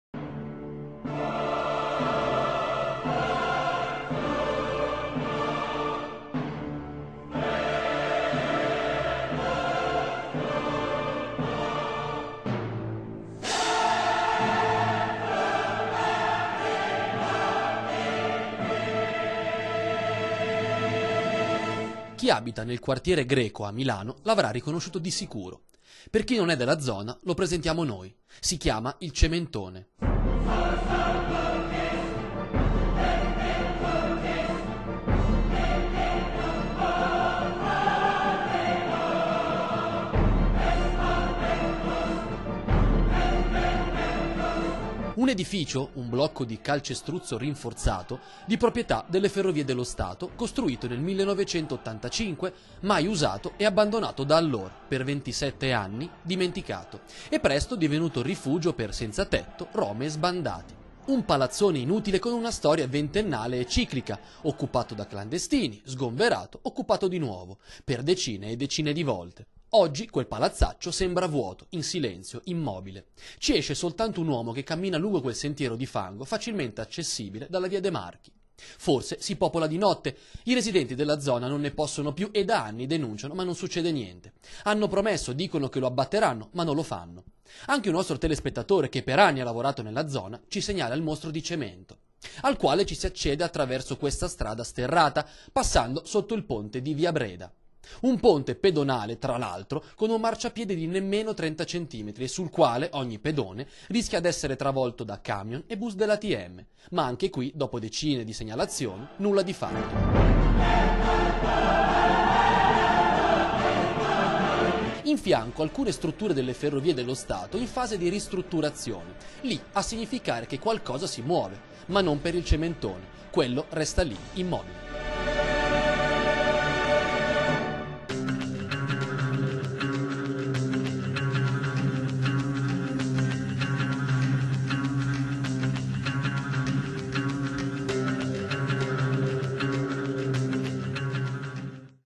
Il "cementone" di Greco è ancora lì...dopo 27 anni
L'edificio delle Ferrovie dello Stato, ribattezzato "cementone", dopo 27 anni è ancora lì. Un ecomostro di cemento costruito nel 1985 e mai utilizzati. Da anni rifugio di zingari, extracomunitari, senza tetto e sbandati.